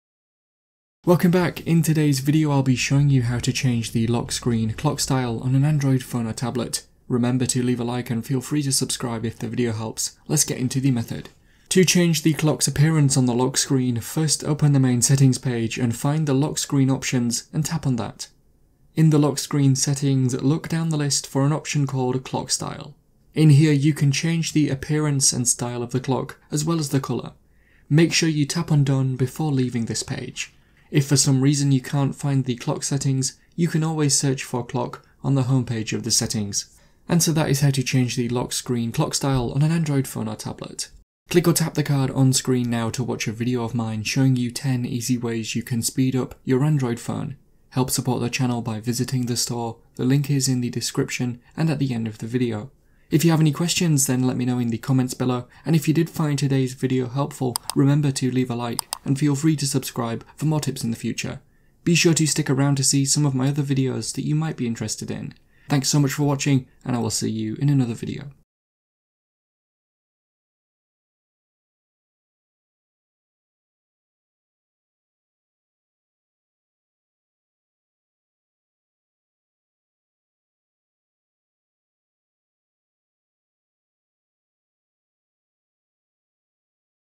How to Change Lock Screen Clock Style on Android Phone or Tablet
Want to know how to change lock screen clock style on Android phone or tablet? This video will show you how to change clock style on Android phone. You might want to know how to change lock screen clock if you want to customise your lock screen. Hopefully the video helps show you how to change clock style on lock screen on Android phones!